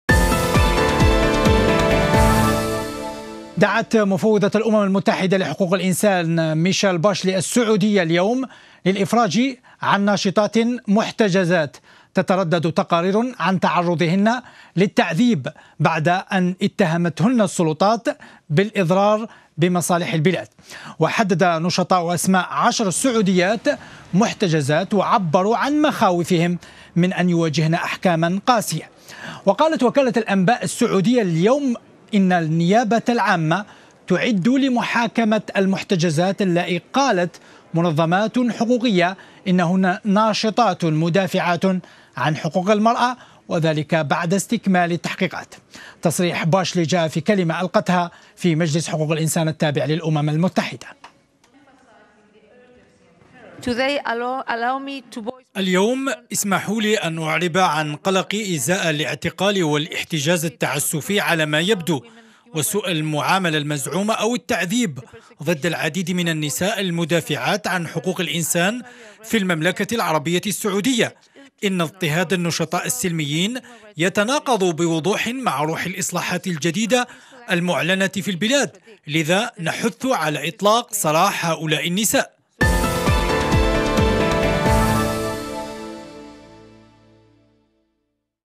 مفوضة حقوق الإنسان تدعو للإفراج عن ناشطات سعوديات
دعت مفوضة الأمم المتحدة لحقوق الإنسان ميشيل باشليه السعودية يوم الأربعاء للإفراج عن ناشطات محتجزات تتردد تقارير عن تعرضهن للتعذيب بعد أن اتهمتهن السلطات بالإضرار بمصالح البلاد.